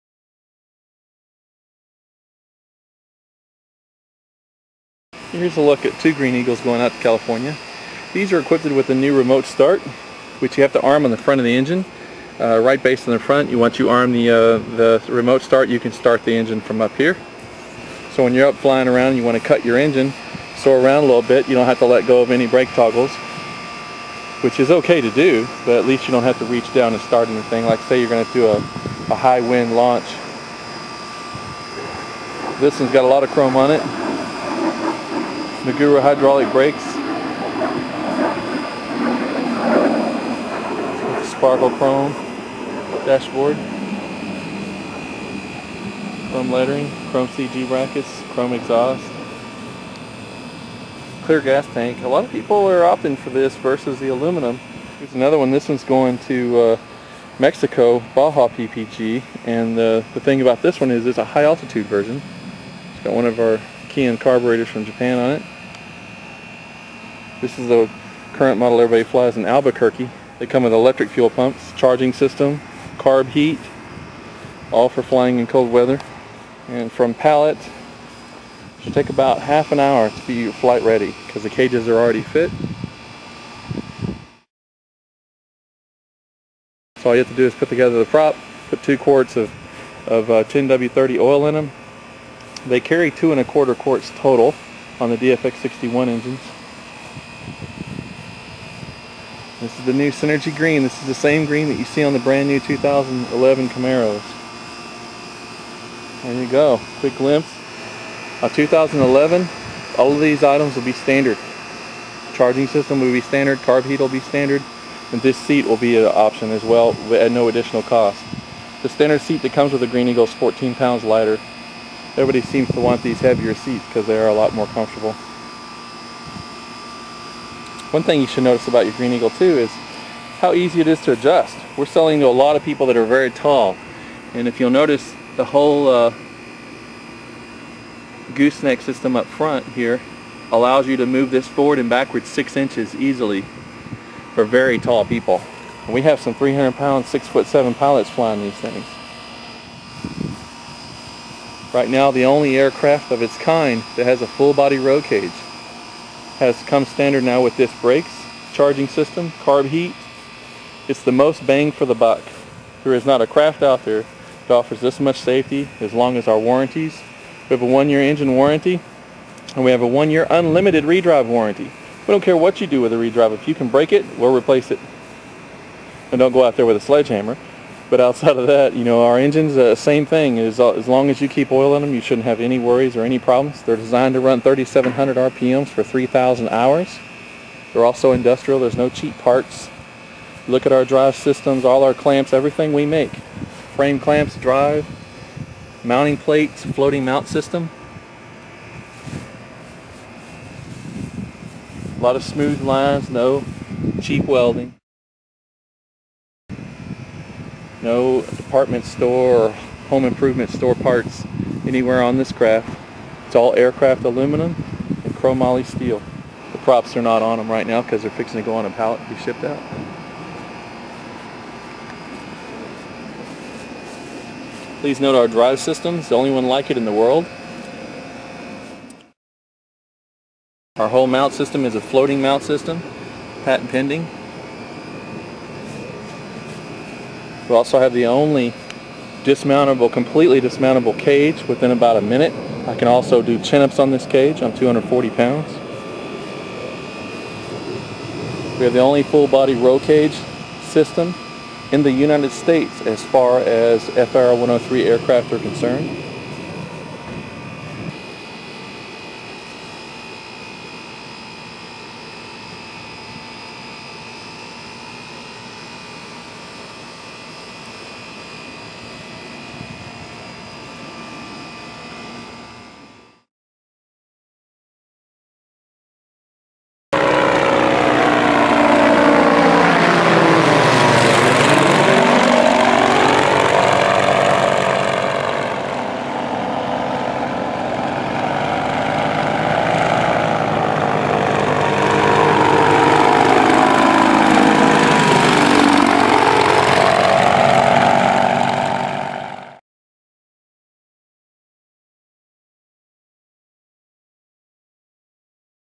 New Eagles #1 personal aircraft
The new 2011 Green Eagle  The worlds #1 personal  Aircraft ,  Best ergo friendly aircraft in the market with the only full body row cage in the industry, 4 point harness, 4 stroke reliability, low fuel usage , extreme power to weigh ratio,  longest warranties, electric start, charging system, and a price below 10k   There is no better time or deal than the Green Eagle.  Each craft is custom made to your specification, custom powdercoat , hydraulic brakes come standard.   Dream It Build It Fly It.  www .FlyGreenEagle. com